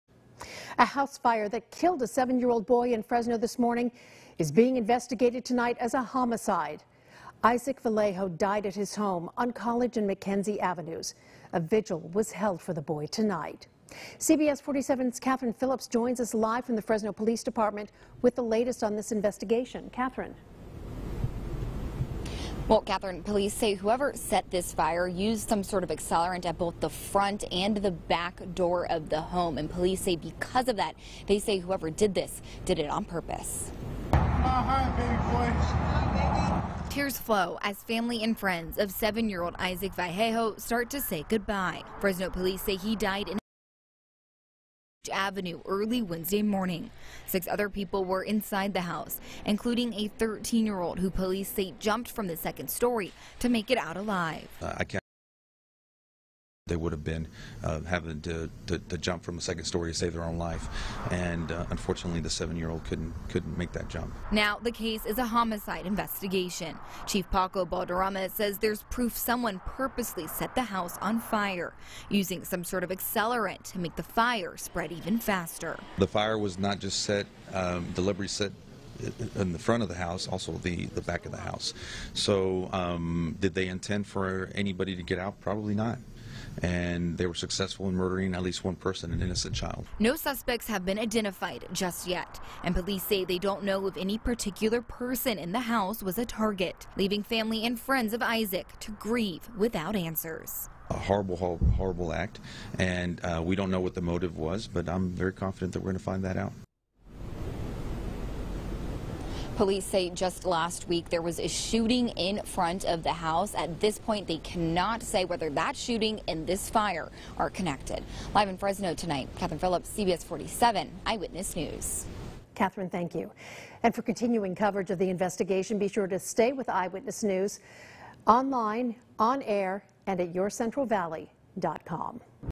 Vigil held for 7-year-old boy killed in Fresno house fire
Tears were flowing as family and friends of Isaac Vallejo gathered to say goodbye to him during a candlelight vigil on Wednesday night.
The Fresno Police Department said the 7-year-old boy was killed in a house fire near College and McKenzie avenues around 4:00 a.m. the same day.
Six other people were inside the house when the fire broke out, including a 13-year-old who police say jumped from the second story to make it out alive.